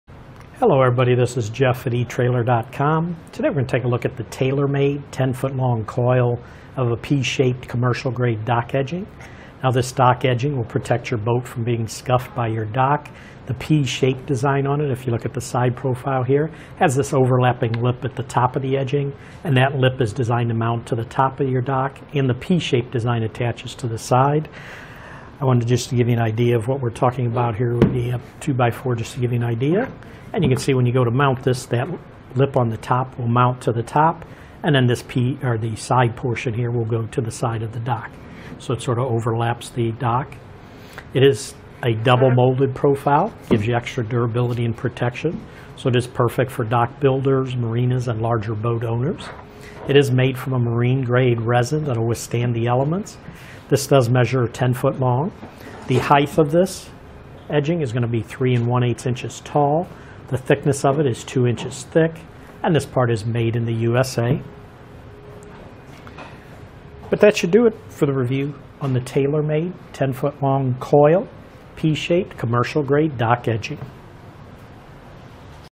etrailer | Taylor Made Commercial Grade Dock Edging Review
Click for more info and reviews of this Taylor Made Boat Dock Bumpers:

Jeff:                                                  Hello everybody, this is Jeff at etrailer.com. Today, we're going to take a look at the tailor-made 10 foot long coil of a P shaped commercial grade dock edging. Now this dock edging will protect your boat from being scuffed by your dock. The P shaped design on it if you look at the side profile here, has this overlapping lip at the top of the edging, and that lip is designed to mount to the top of your dock and the P shaped design attaches to the side.I want to just give you an idea of what we're talking about here with the 2x4, just to give you an idea. And you can see when you go to mount this, that lip on the top will mount to the top, and then this P or the side portion here will go to the side of the dock, so it sort of overlaps the dock. It is a double molded profile, gives you extra durability and protection.

So it is perfect for dock builders, marinas, and larger boat owners. It is made from a marine grade resin that'll withstand the elements. This does measure 10 foot long. The height of this edging is going to be three and one eighths inches tall. The thickness of it is two inches thick, and this part is made in the USA.

But that should do it for the review on the tailor-made 10 foot long coil P shaped commercial grade dock edging.